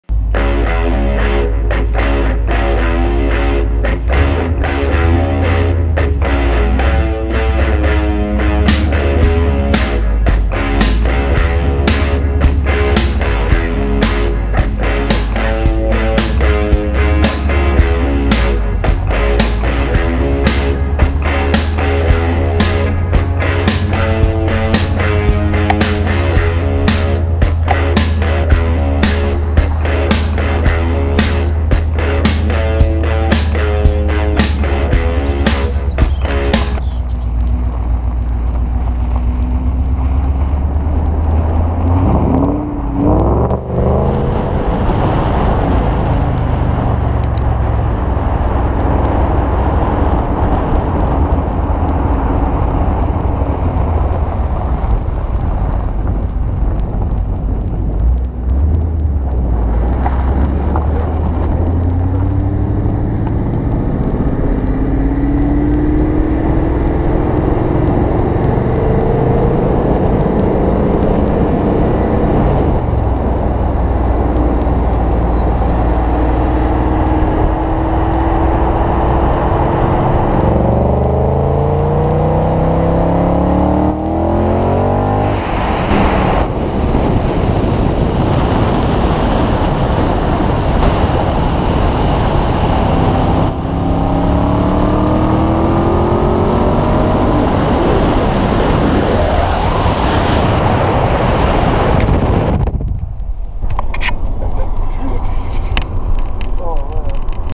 Killing time with the Stang
Wanted to test the gopro 4 black. What better way than in the stang...